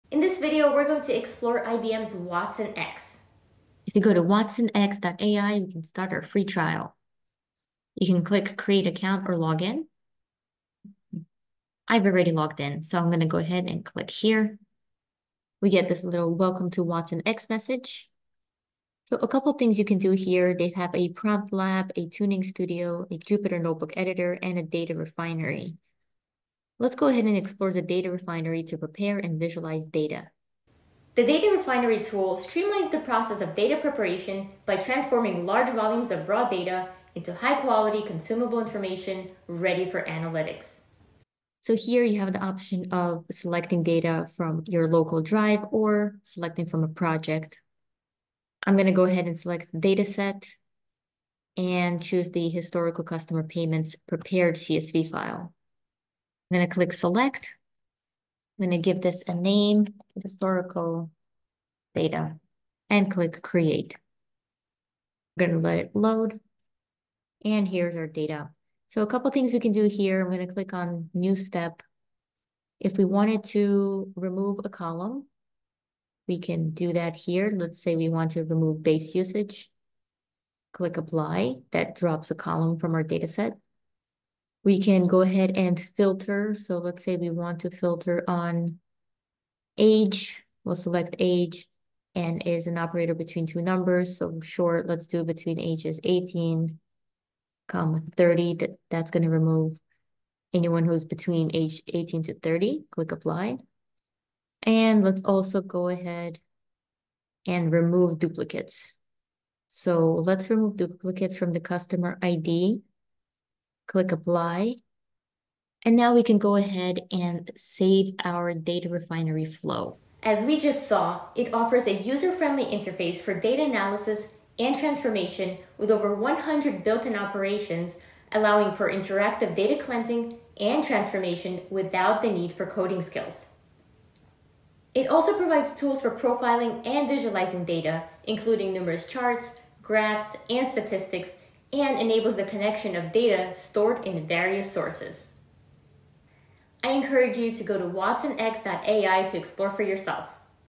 IBM watsonx data refinery
I encourage anyone that wants to dip their feet into AI WATER - to check out IBM watsonx - you can try it out for free
Here’s a glimpse into the Data Refinery feature of the platform.